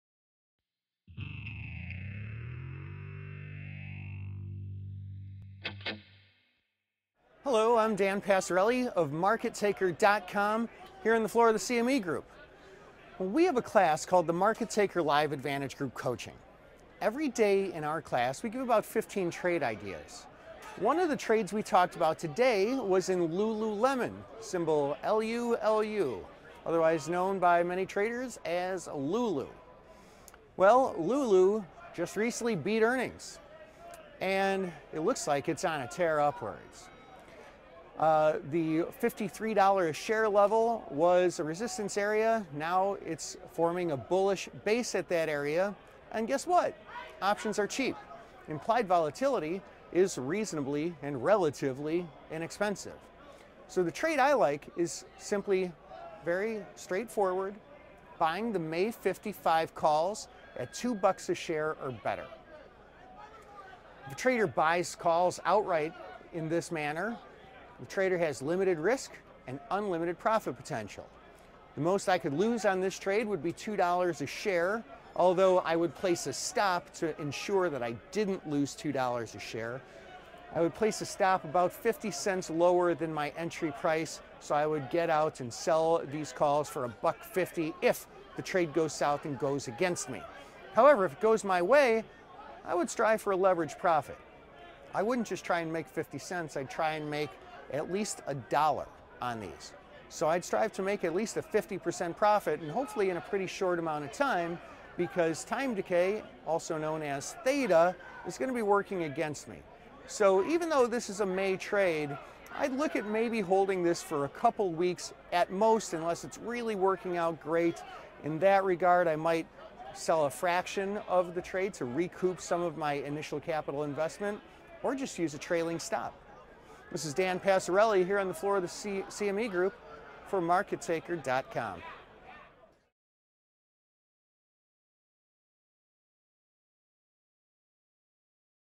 LULU Beat Earnings
Dan talks about trade Lululemon. Lulu just recently beat earnings.